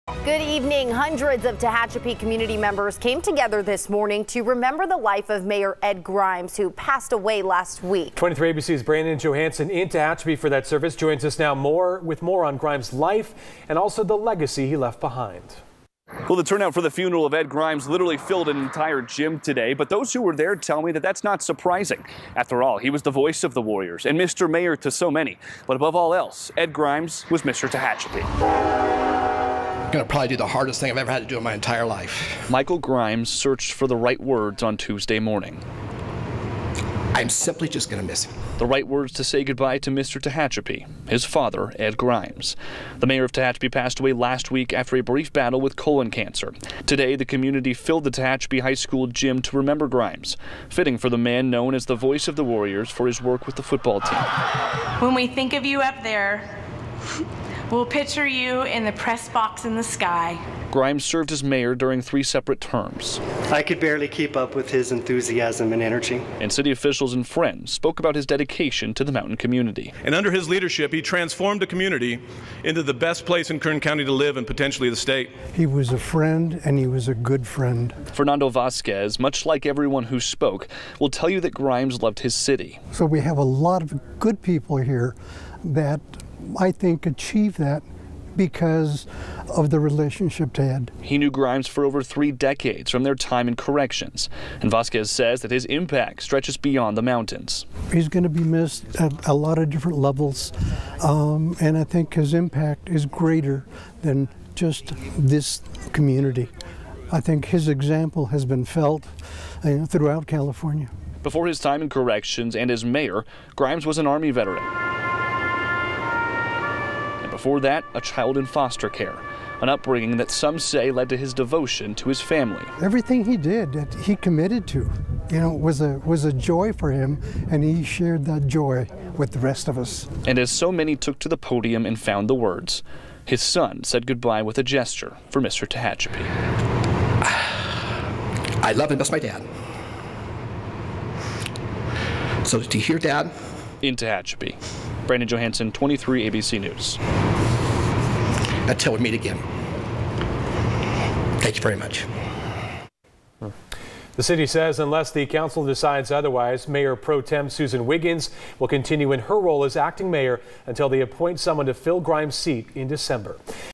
Tehachapi gathers to remember Ed Grimes
Hundreds gathered in Tehachapi to say goodbye to "Mr. Tehachapi," Mayor Ed Grimes during a memorial service on Tuesday at the local high school gym.